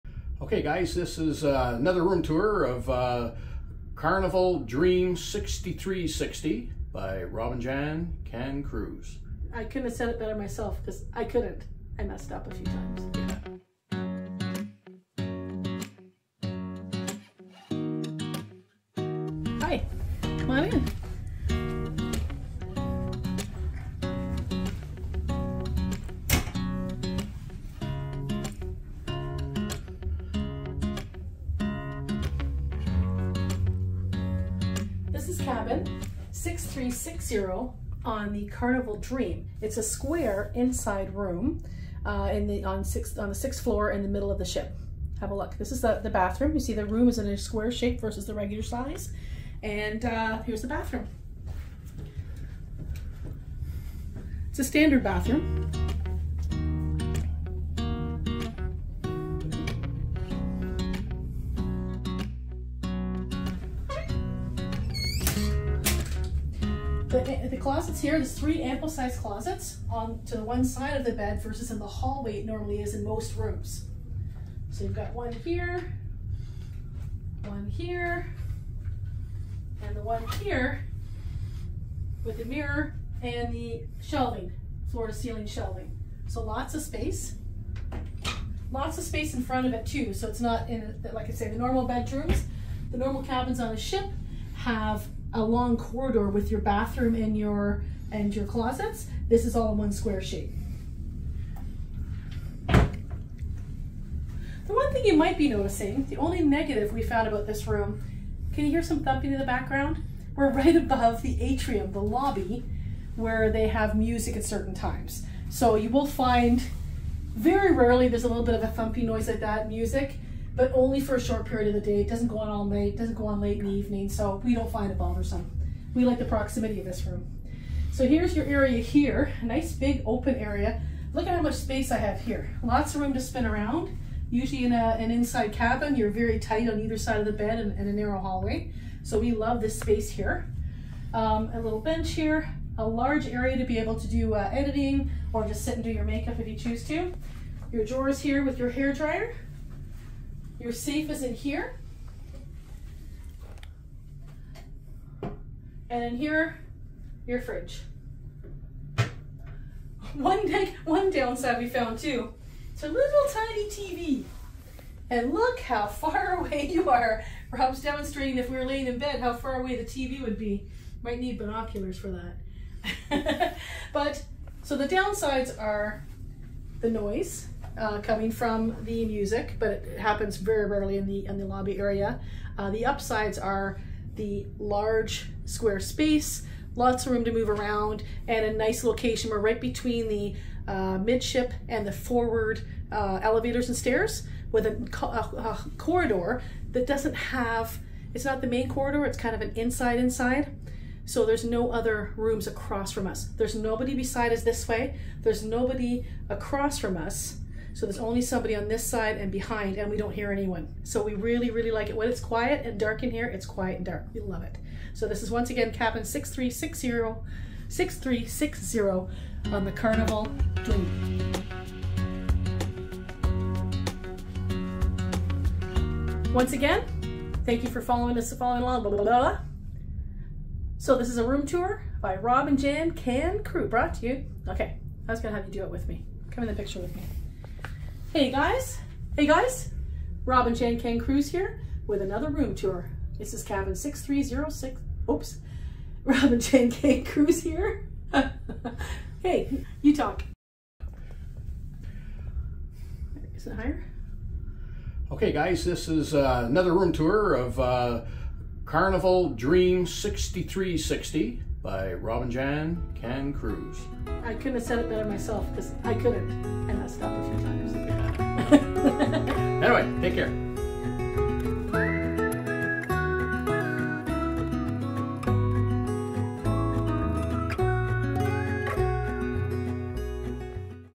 CARNIVAL DREAM INSIDE CABIN TOUR - ROOM 6360 - PROS & CONS
Welcome to Cabin 6360, an inside cabin on Carnival Dream!
Would you stay in this cabin? The location is great. The square configuration is spacious for an inside cabin...but it isn't perfect!
We are Rob & Jan Can Cruise, and when we aren't living in our self-converted Van Home, we are sailing all over on cruise ships.